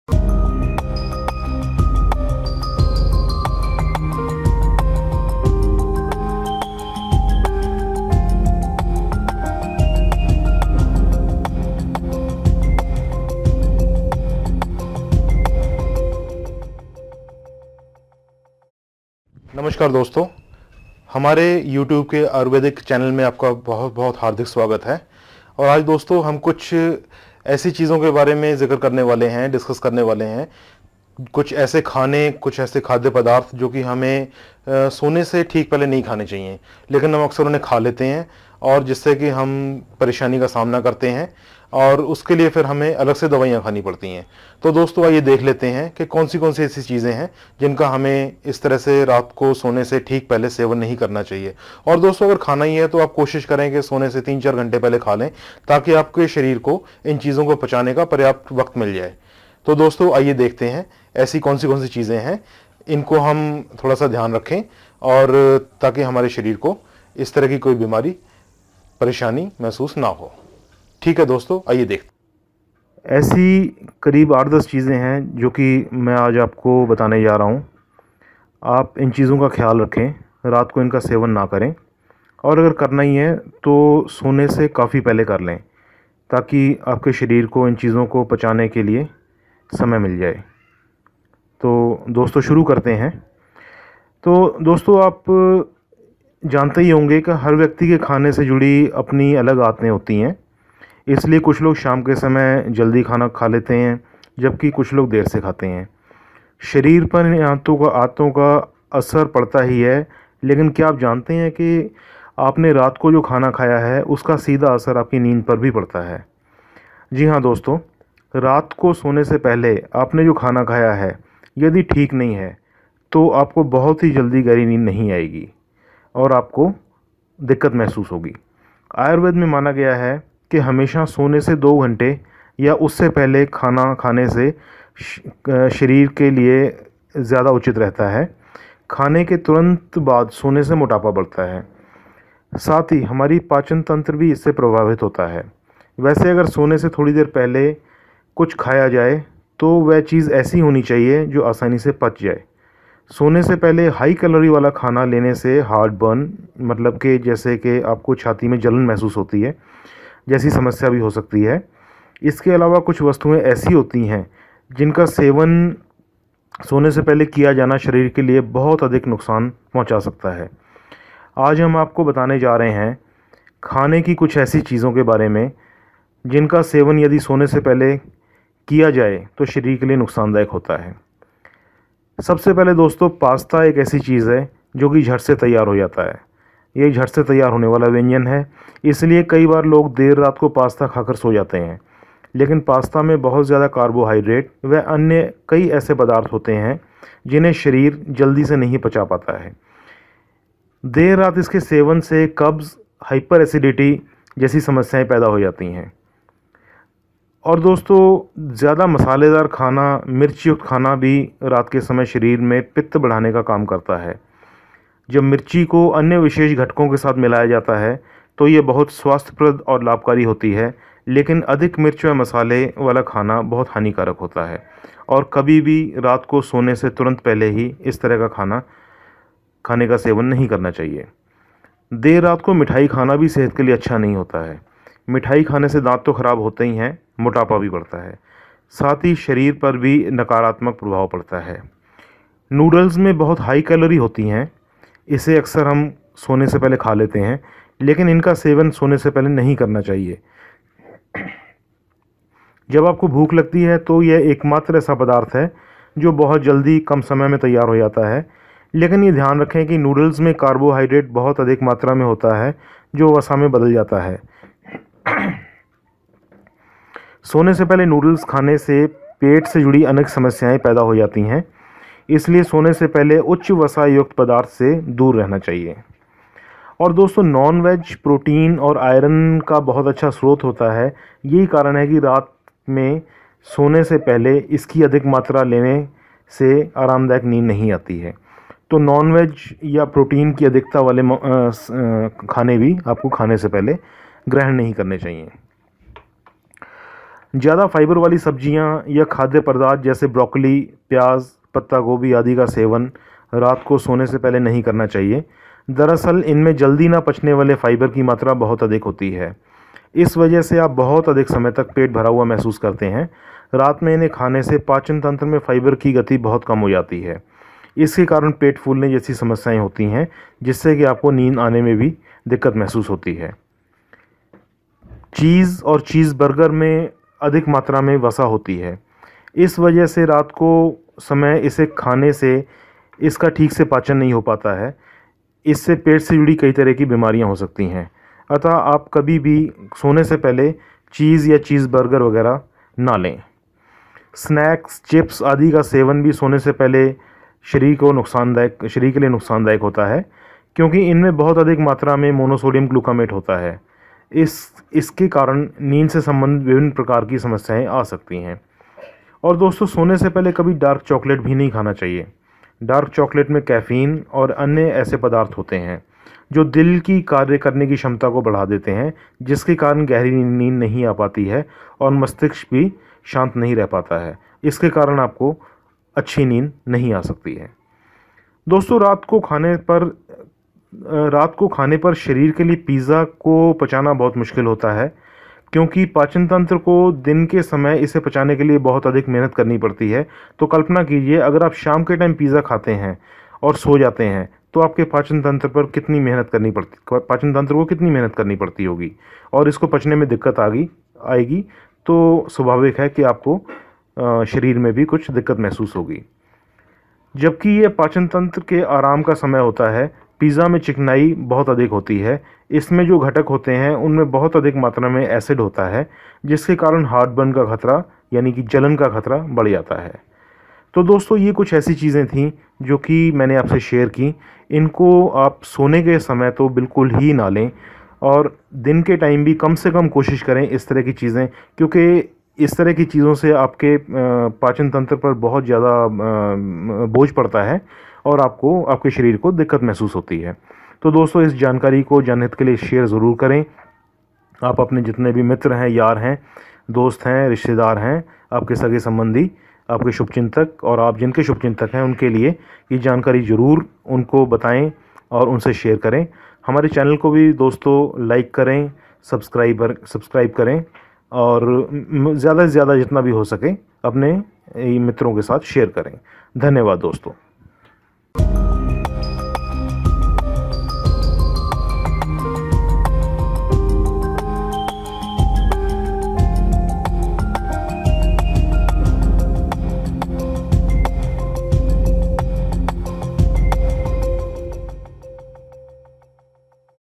रात को नहीं खानी चाहिए ये 10 चीजें - Only Ayurved
Foods to be avoided before sleeping.